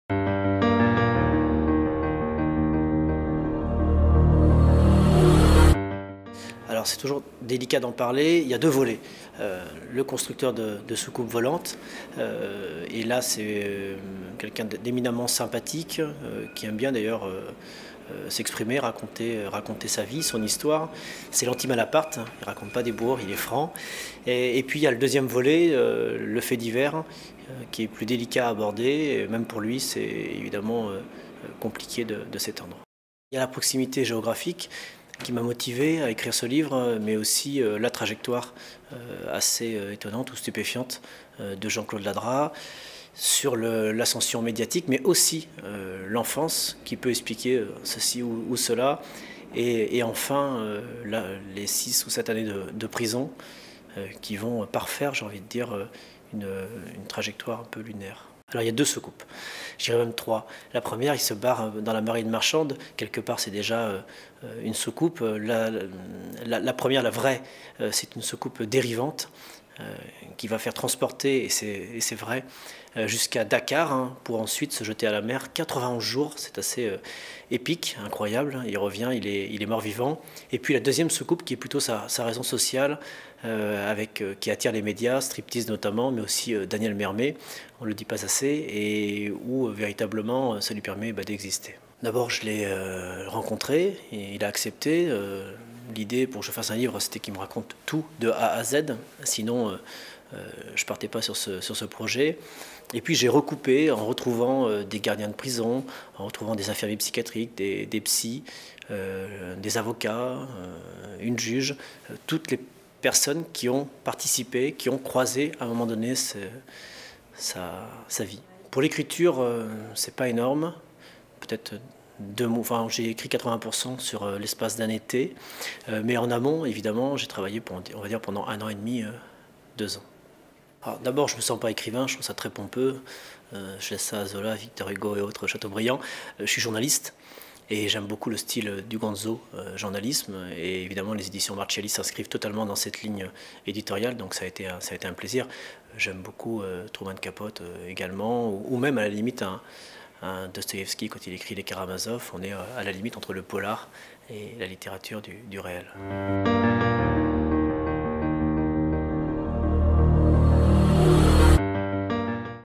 Jean-Charles Chapuzet - Mauvais plan sur la comète
Jean-Charles Chapuzet vous présente son ouvrage "Mauvais plan sur la comète : l'étrange odyssée d'un Haut Saintongeais" aux  éditions Marchialy. Rentrée littéraire 2018.